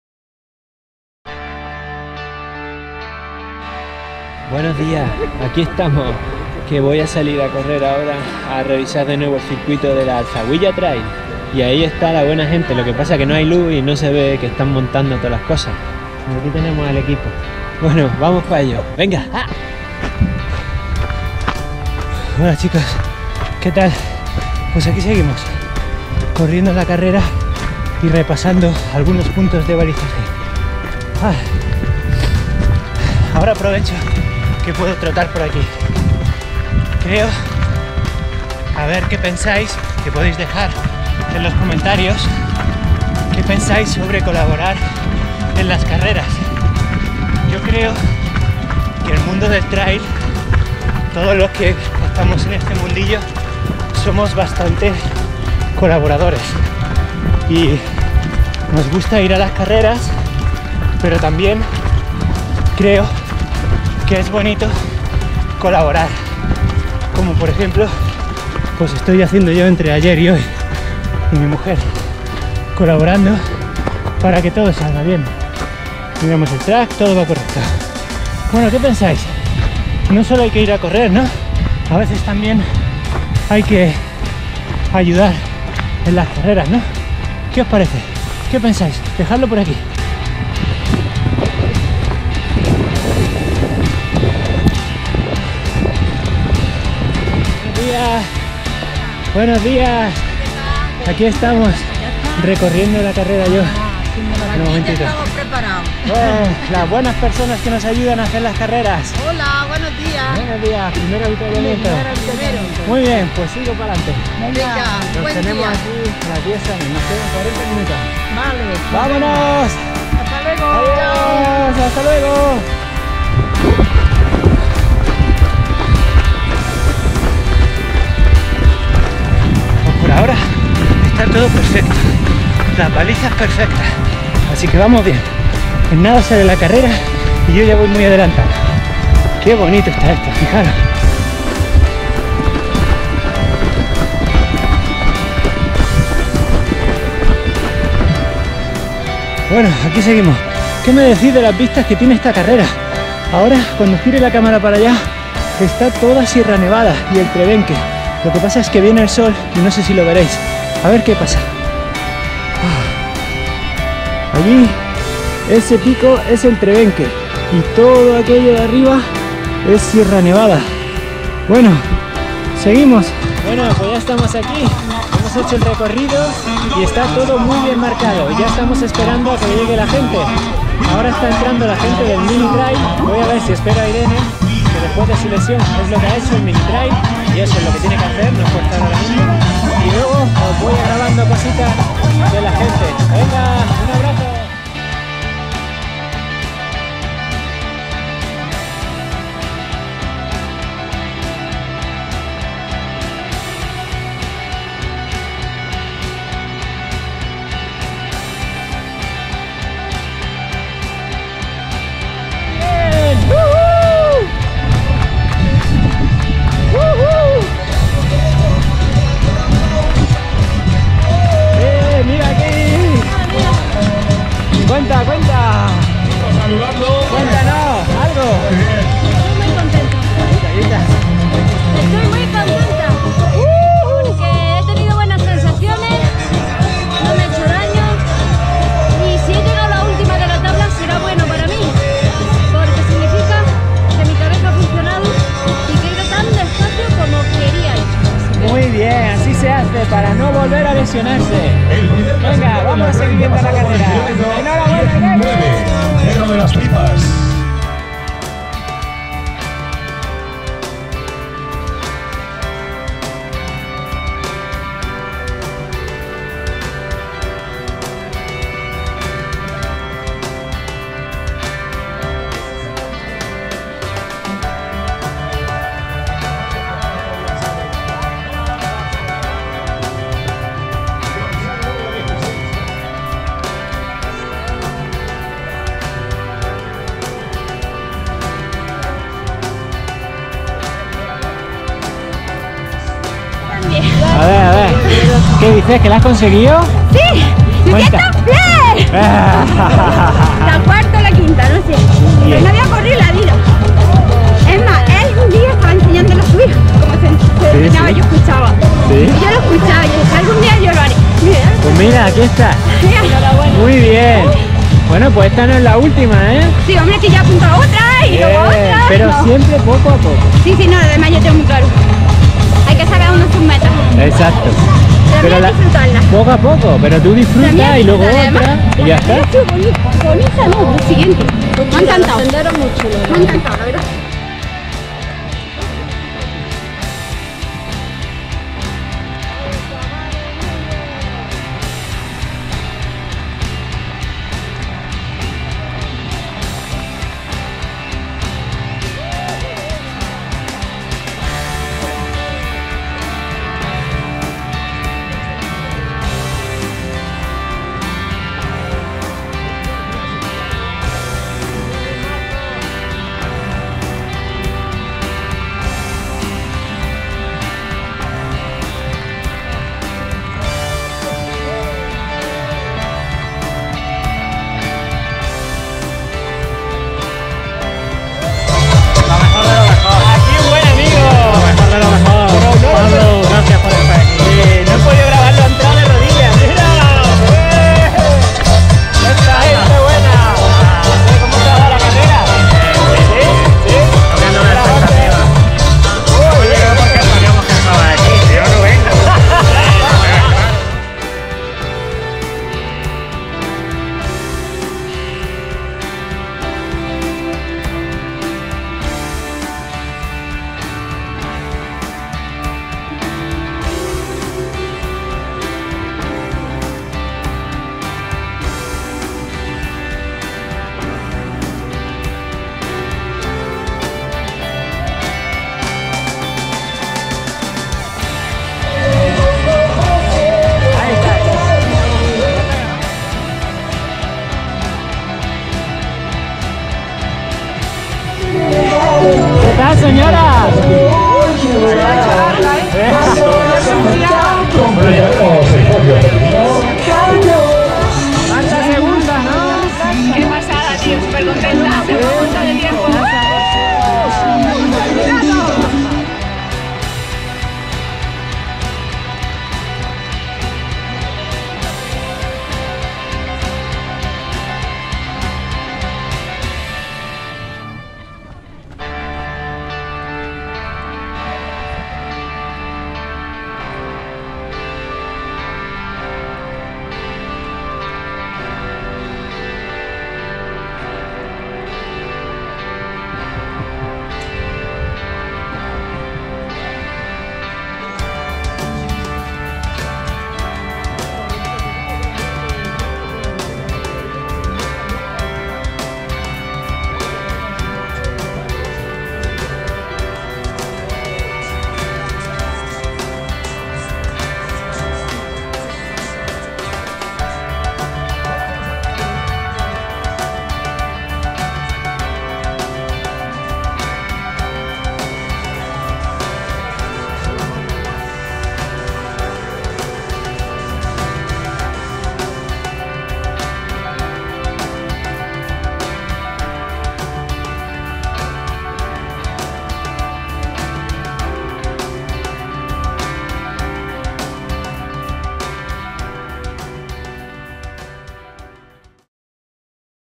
VBLOG 06 AL-ZAWIYA TRAIL 2017 ECHANDO UNA MANO Y EL REGRESO DE IRENE
Echamos una mano en el balizaje y la revisión de la AL-ZAWIYA Trail de 2017, una carrera en las faldas de Sierra Nevada

FRONTAL ARMYTEK WIZARD V3:

ZAPATILLA ALTRA SUPERIOR 3.0:

GAFAS SALICE 3047ITA:

MOCHILA ULTRASPIRE ALPHA3.0 EN COLOR NEGRO

MOCHILA ULTRASPIRE ALPHA3.0 EN COLOR ROJO